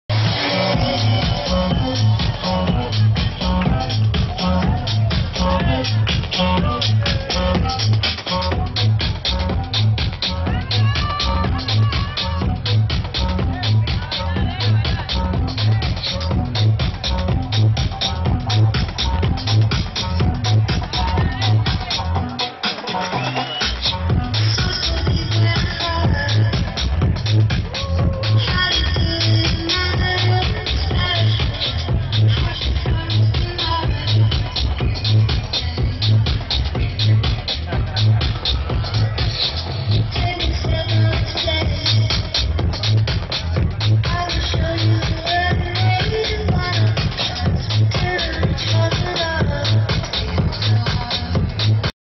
Martin Eyerer playing last record before closing@Goa Madrid 19.02.2012.mp4
superb art is hard tune. Final record before closing the night played by Martin Eyerer on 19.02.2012 @ Goa Madrid carnaval party